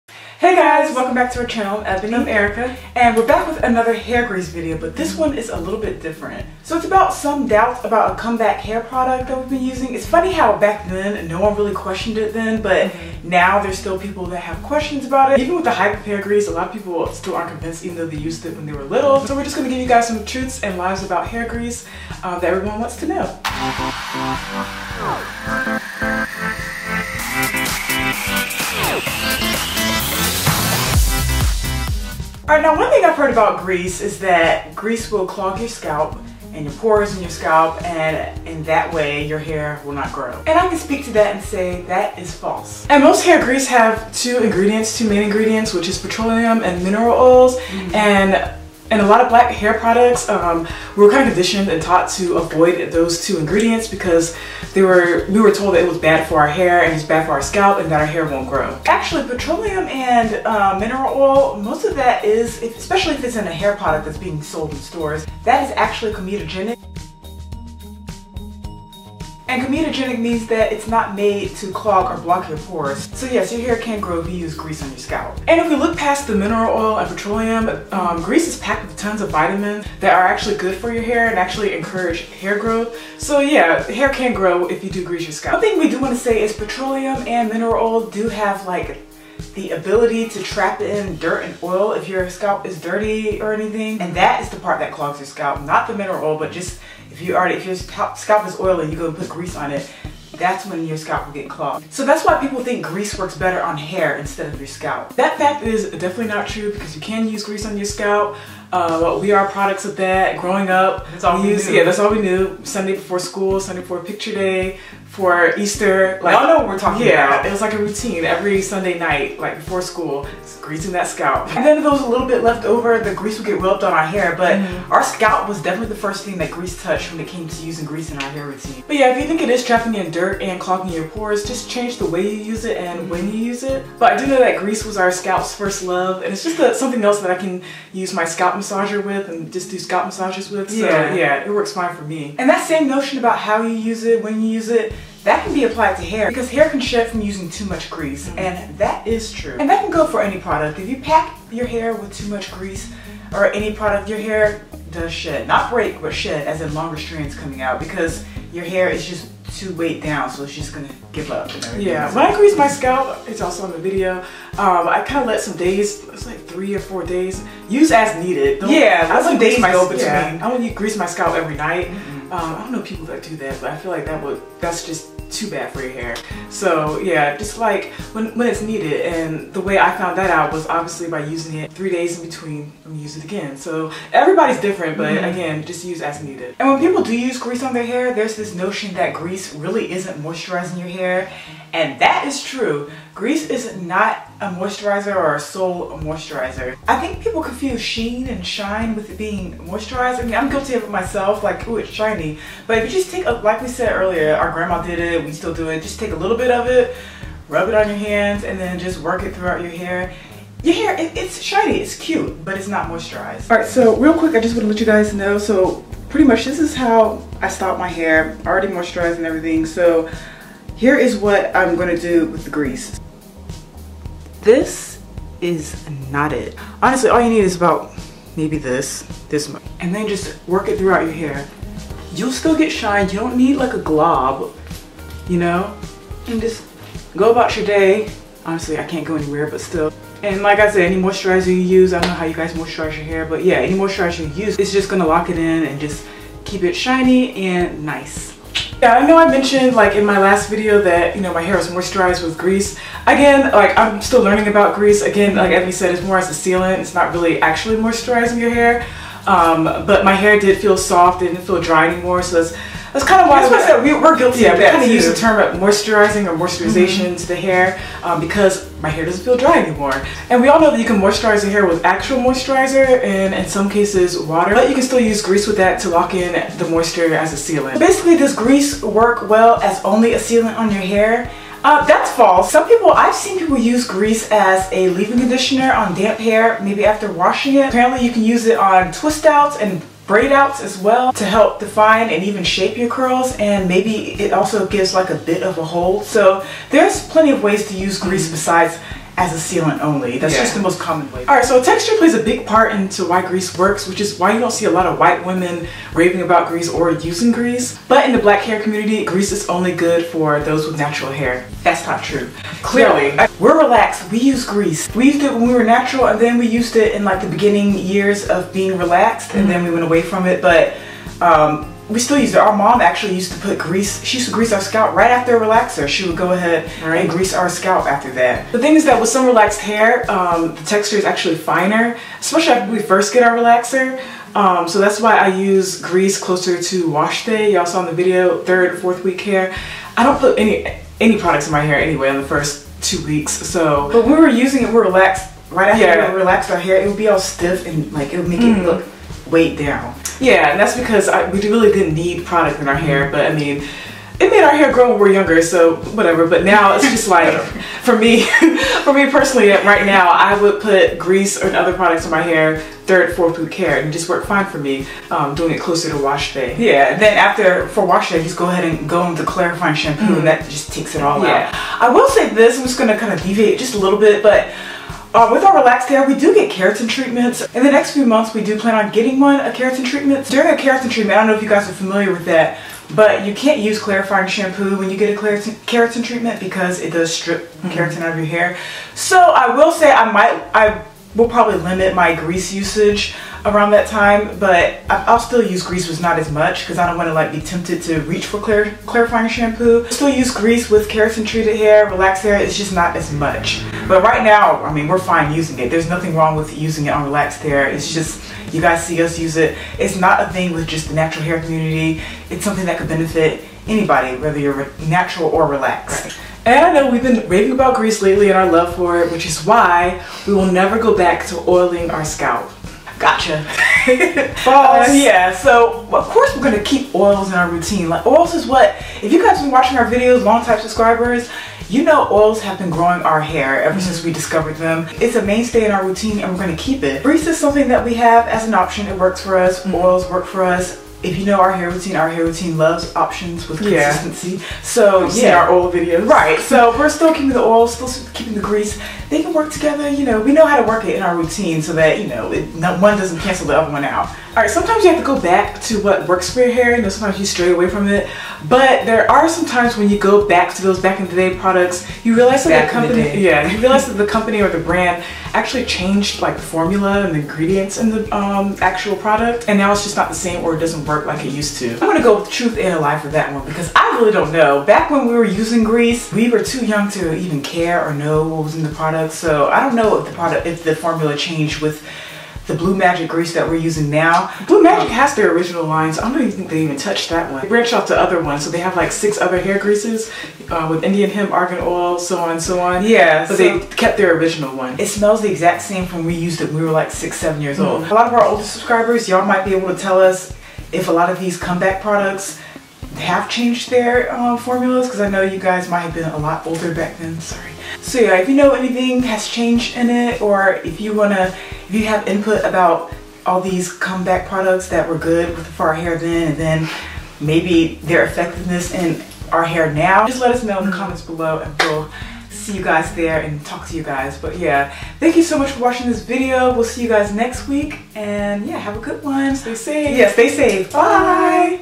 TRUTH AND LIES ABOUT HAIR GREASE: DOES IT STOP HAIR GROWTH?
So does hair grease really grow hair? Does grease clog your scalp with its mineral oil and petroleum that's bad for hair ?  Is hair grease better for natural hair or relaxed hair? Does grease really moisturize your hair. We recently got back to basics with blue magic hair grease on our relaxed hair, but we really want to see what is it about grease or any secrets that's in grease that make it maybe too good to be true.

-BLUE MAGIC GREASE WE USE:

*SEND US MAIL:  Eboni and Erica
                               18960 Ventura Blvd #132